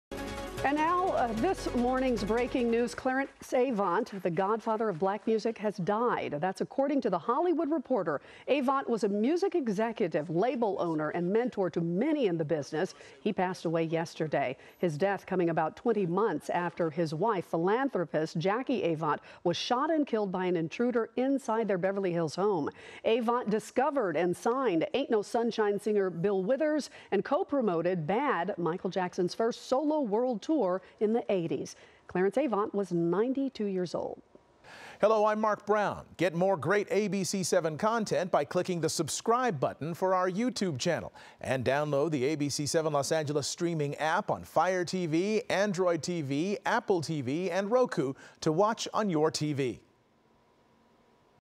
Clarence Avant, 'Godfather of Black Music,' dies at 92
Clarence Avant, the music executive known as the "Godfather of Black Music," has died at the age of 92, according to his family.